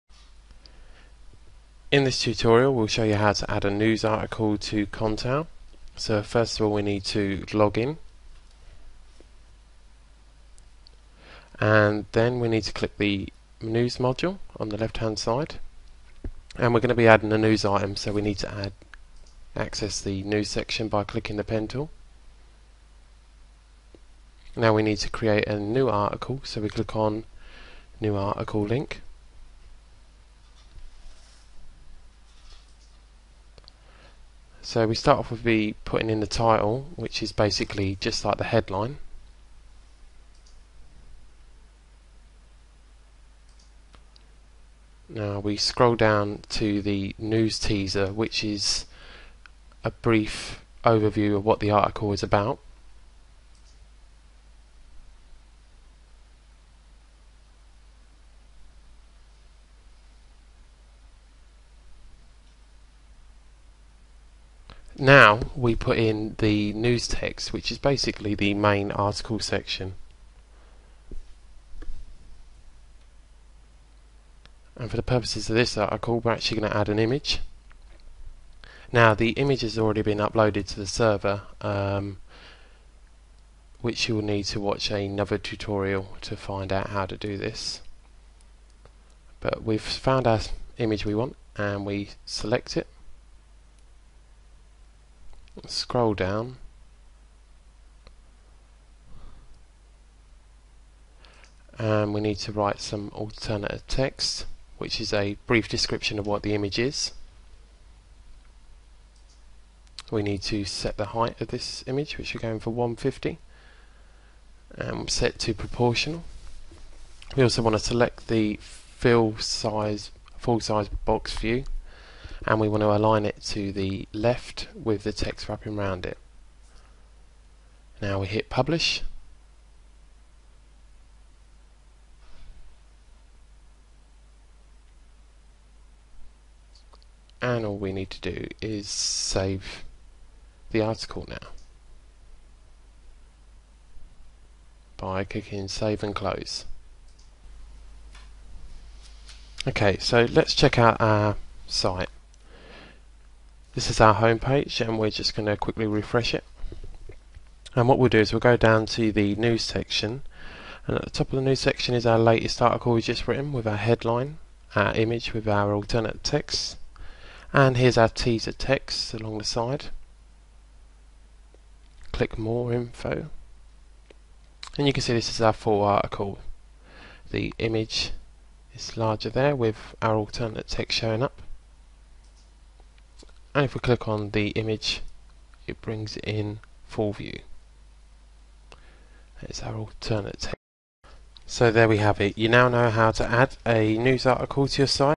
Contao Tutorial: How to add a news item
In this tutorial we will show you how to add a news article into Contao.